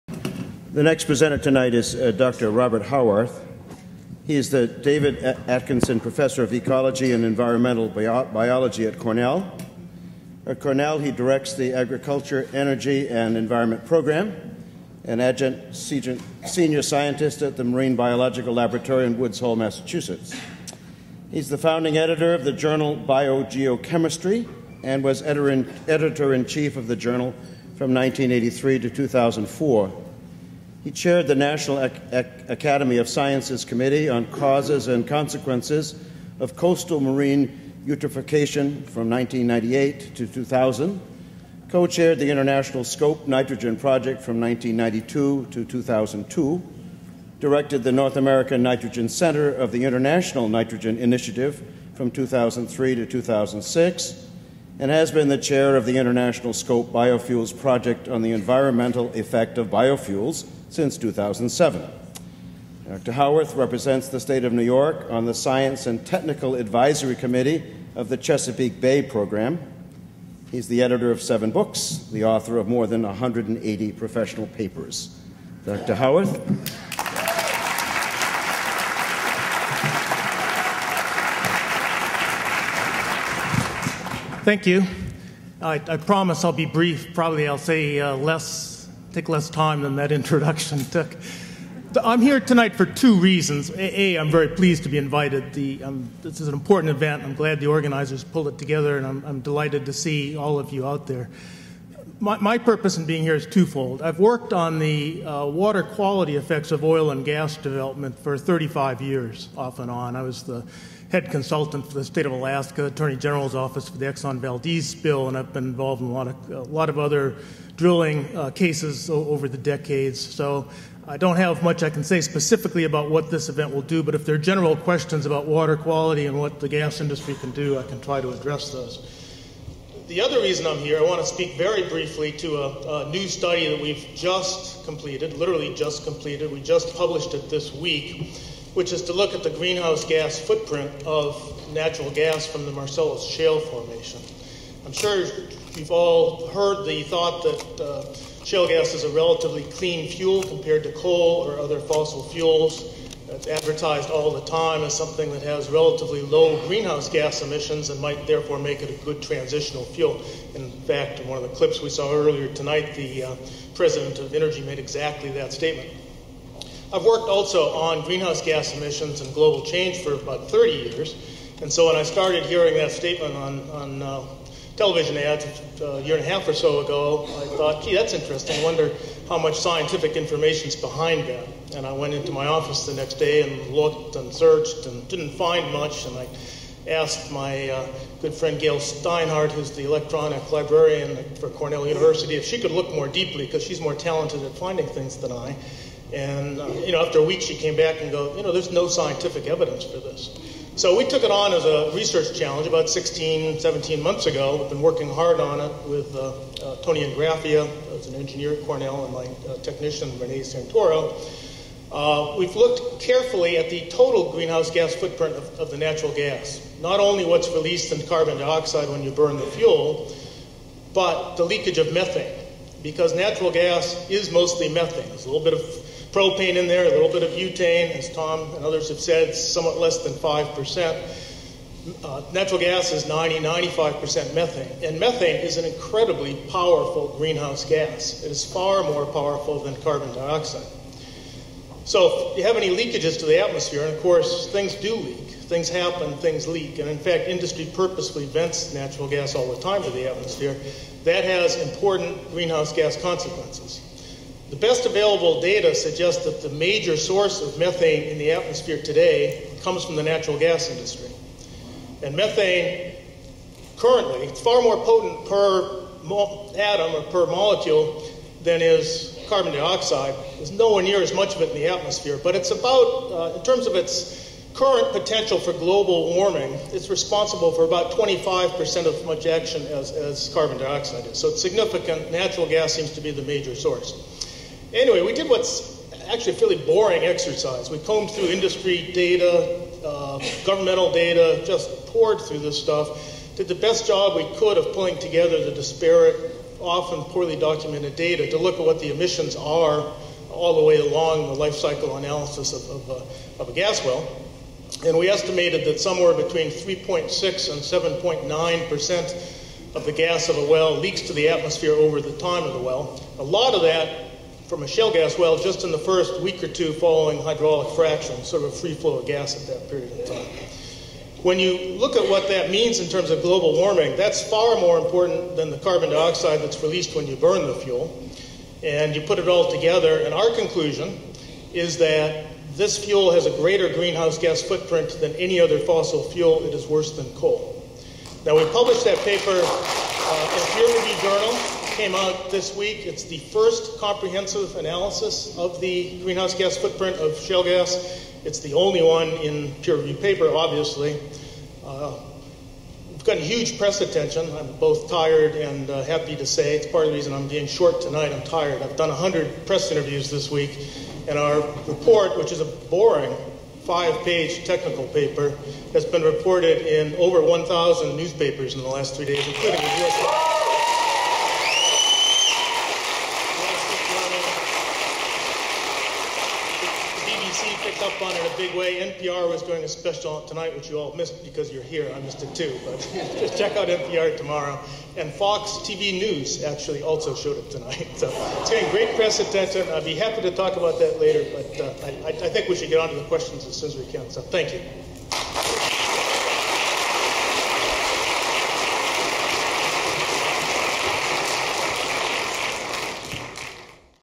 5-Robert Howarth, Ph.D, ecologist, environmental biologist,
SENECA AT A CROSSROADS: Large Scale LPG Storage vs. Tourism, Wineries, Peace and Quiet - Watkins Glen, NY. ; A Forum to inform the public about how out-of-state Inergy LP plans to store hundreds of millions of gallons of LPG (liquefied petroleum gas) in salt caverns right next to and under Seneca Lake. They also plan to store nearly a hundred million gallons of very salty Brine in a 14 acre open pit on the steep slope above the lake.